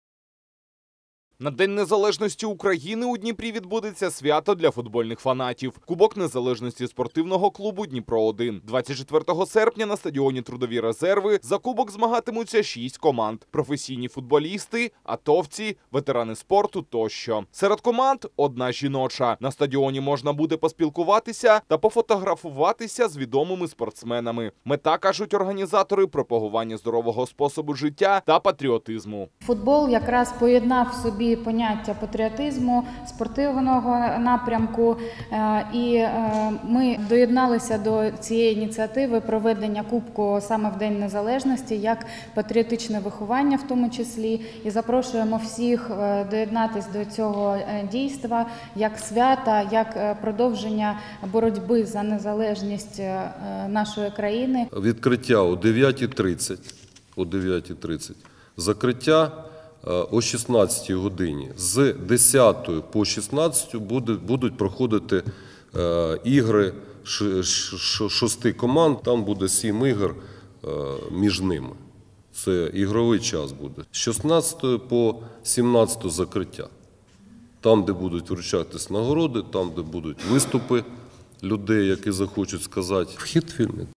Кубок Незалежності серед команд спортивного клубу Дніпро-1 відбудеться у Дніпрі 24 серпня
На День незалежності України у Дніпрі відбудеться свято для футбольних фанатів — Кубок Незалежності спортивного клубу Дніпро-1. 24 серпня на стадіоні Трудові резерви за кубок змагатимуться 6 команд: професійні футболісти, АТОвці, ветерани спорту тощо. Серед команд - одна жіноча. На стадіоні можна буде поспілкуватися та пофотографуватися з відомими спортсменами. Мета, кажуть організатори, пропагування здорового способу життя та патріотизму.